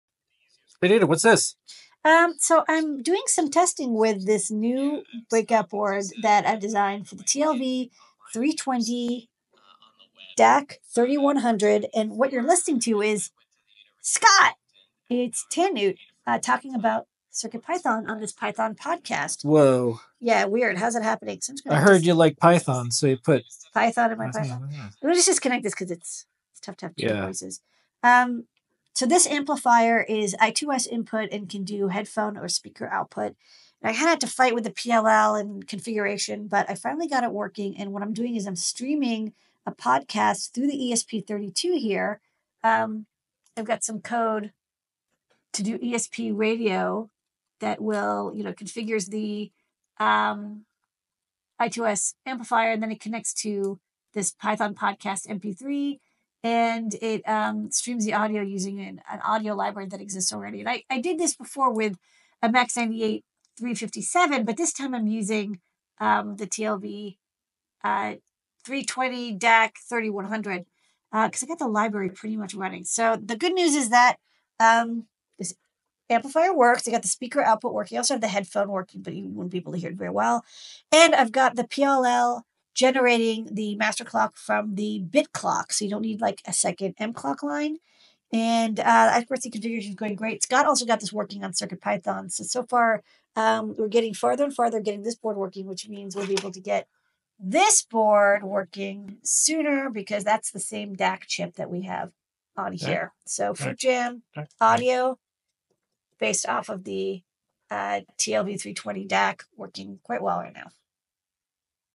TLV320DAC3100 first bops 🔊🎶💃🕺
OK, after many hours spent with Claude on writing a driver for the TLV320DAC3100 we finally have it configured using our driver, and playing an MP3 stream on this ESP32. This I2S DAC has a particularly complex PLL and audio-routing system, so it's not one where you can just pipe in I2S data and have it magically play. One nice thing we got working on is the MCLK, which is generated from the BCLK, so it'll work great with anything from an Arduino-compatible to a single-board computer like Raspberry Pi. We're hoping to get the headphone detection working next so that we can turn off the amp when the headphone is plugged in. Also, it should be able to control the volume from the headset buttons. Also, we want to get the internal beep generator going so we can make tones separate from the audio stream for UI notifications.